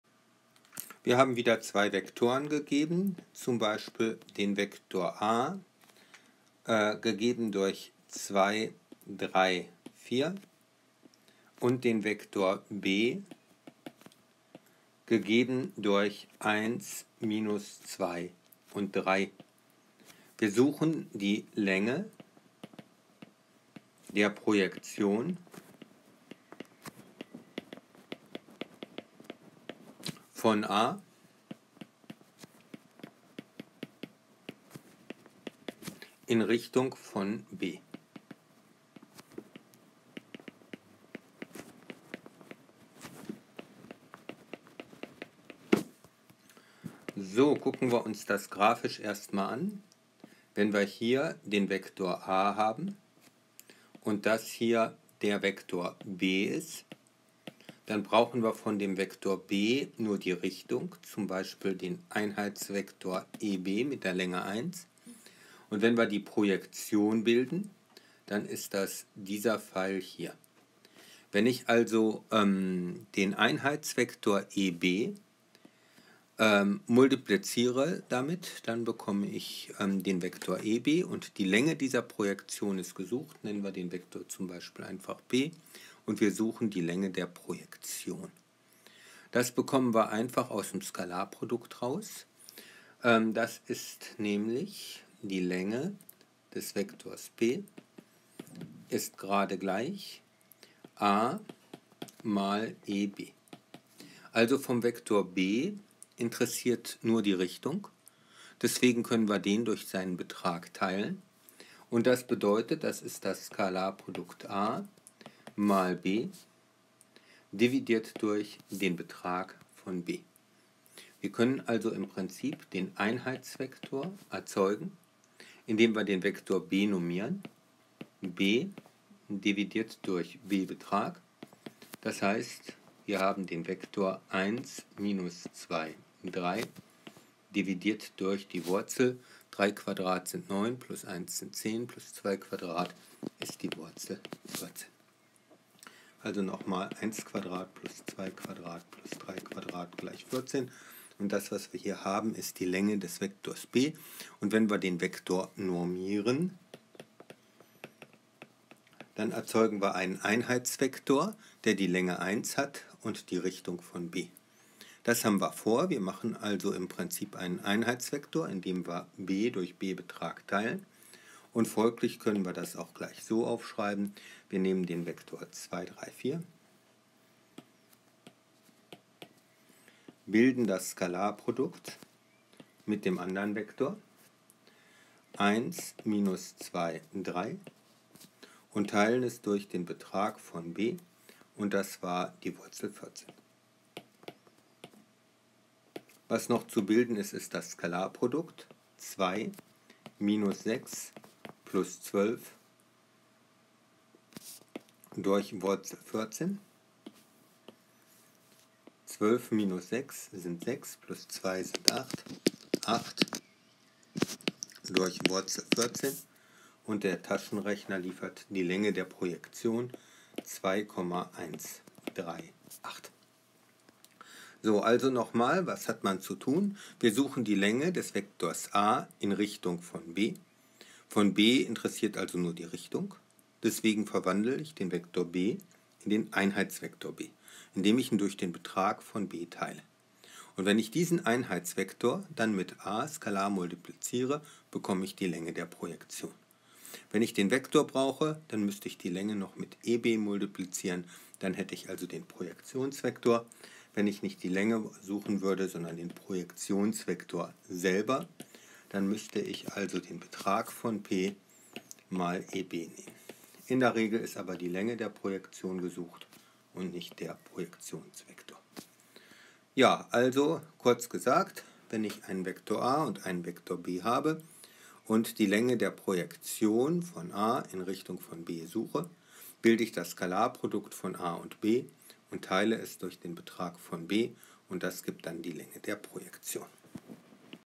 Theorie: Länge der Projektion eines Vektors a in Richtung von b
Beim Vektor b interessiert also nur die Richting, deswegen wird der Vektor inormiert, so dass er die Länge 1 hat, indem man durch seinen Betrag teilt. Das Skalarprodukt a mal Einheitsvektor in Richtung von b ergibt die Länge der Projektion, also Skalarprodukt a b bilden und durch b Betrag teilen. Nimmt man diesen Betrag mal dem Einheitsvektor eb = Vektor b durch Betrag b, kann man die Projektion auch als Vektor bekommen.